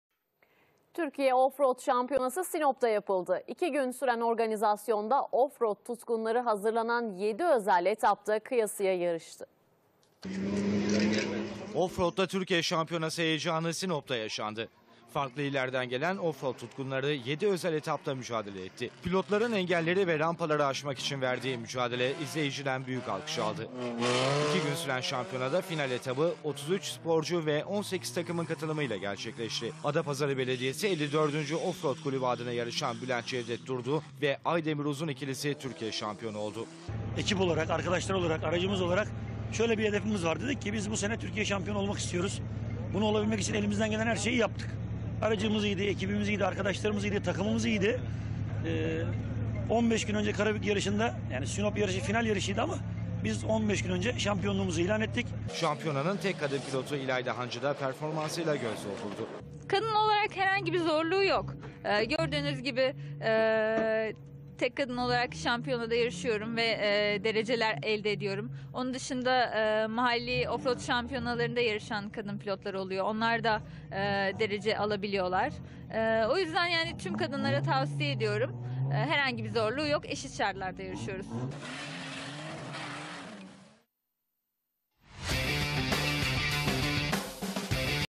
TÜRKİYE OFF-ROAD ŞAMPİYONASI SONA ERDİ
Türkiye Off-Road Şampiyonası'nın final etabı, Sinop'ta yapıldı. Nefes kesen yarışlara vatandaşlar yoğun ilgi gösterdi. Şampiyonanın final etabında Bülent Cevdet Durdu ve Aydemir Uzun ikilisi, Türkiye şampiyonu oldu.